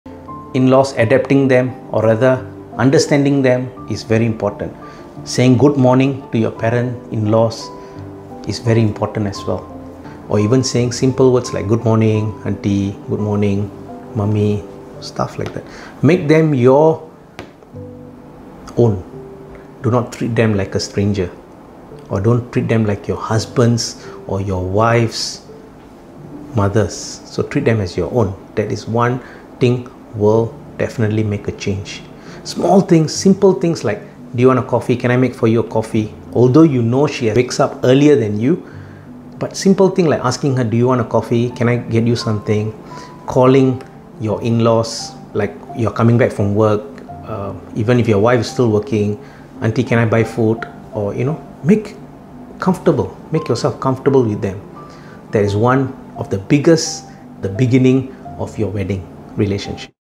Understanding In-Laws
Enjoy our first as we give you tips on how you can understand your in-laws better.